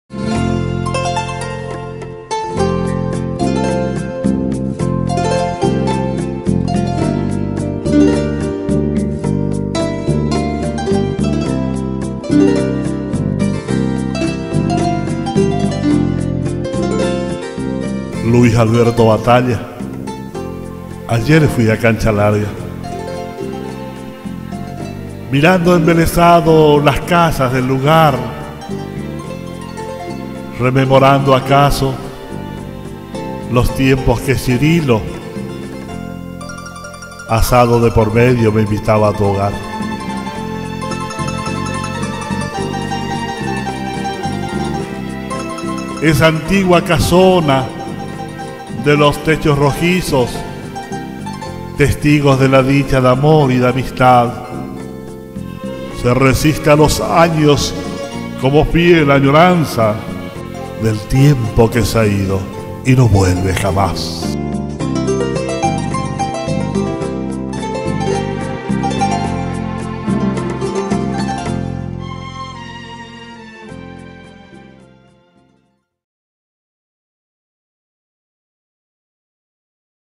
LA VIEJA CASONA.Poema de Fernando Zayas,Luis Batalla tv.mpg
Poema de Fernando Zayas y el recuerdo a mi padre Cirilo Batalla.La vieja casona esta ubicada sobre el camino que va a Cancha Larga.en el barrio La Ralera de La Leonesa Chaco.y su construccion data de la decada del 20.originalmente de la familia de mi abuelo Don Luis Cussigh.donde instalo unos de los primeros almacenes de ramos generales y el primer surtidor de combustibles de la zona.Luego de mi tio Nemecio Reynoso.con la venta de repuestos del automotor y por ultimo adquirida por mi padre Cirilo Batalla en 1984.Actualmente es mi hogar.edicion;Luis Alberto Batalla septiembre 2011.POEMA DE FERNANDO ZAYAS:La vieja casona: Luis Alberto Batalla..ayer fui a "Cancha Larga",mirando embelezado las casas del lugar,rememorando acaso, los tiempos que Cirilo,asado de por medio, me invitaba a tu hogar, esa antigua casona de los techos rojizos,testigos de la dicha de amor y de amistad,se resiste a los años como fiel añoranza del tiempo que se ha ido y no vuelve jamas...